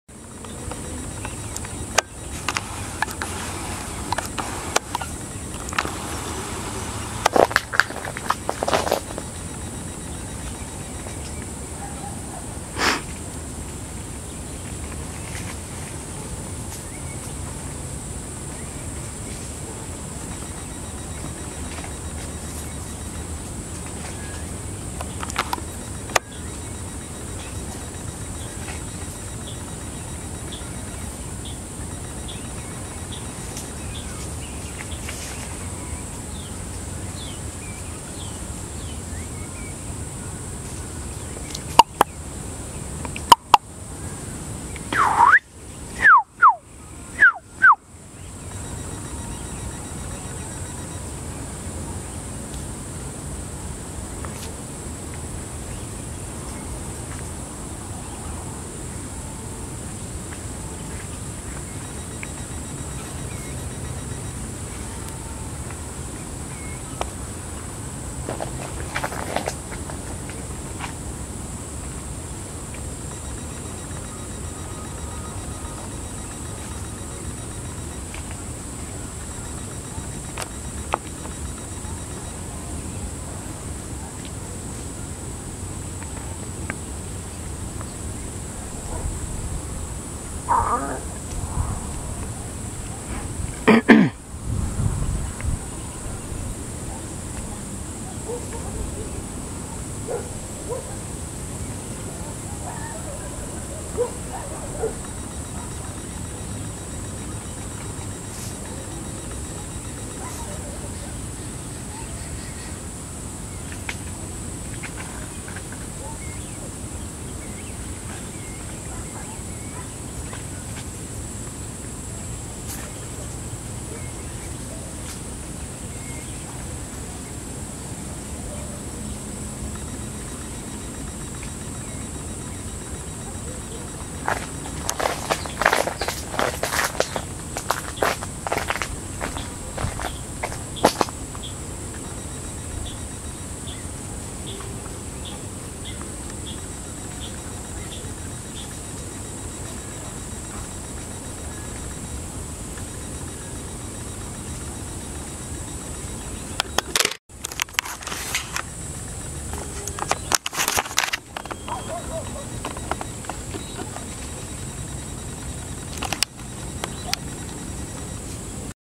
Knysna Touraco in Mount Pleasant, Port Elizabeth, South Africa
A pair of Knysna Touracos in Mount Pleasant, Port Elizabeth, South Africa